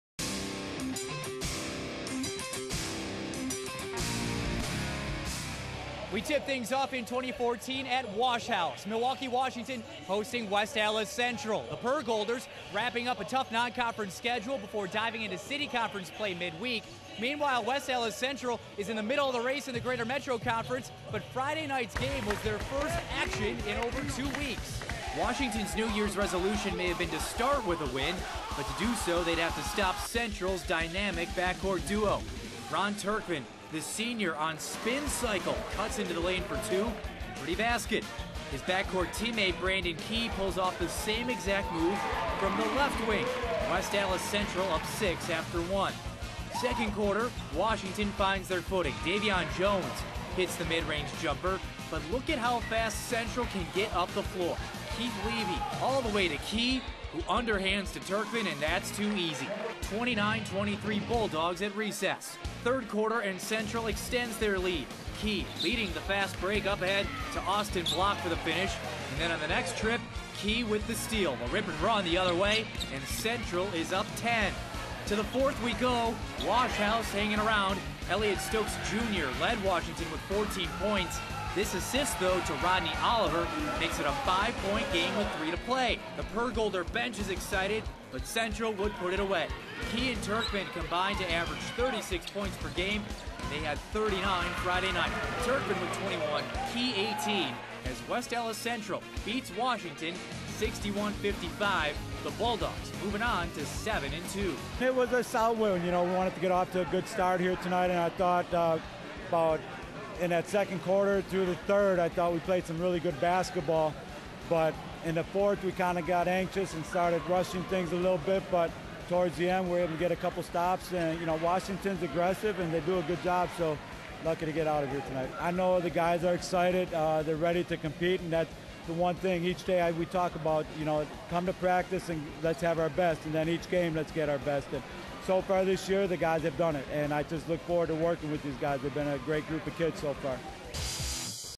Game of the week, 1/5/14
West Allis Central @ Milwaukee Washington - Boy's Basketball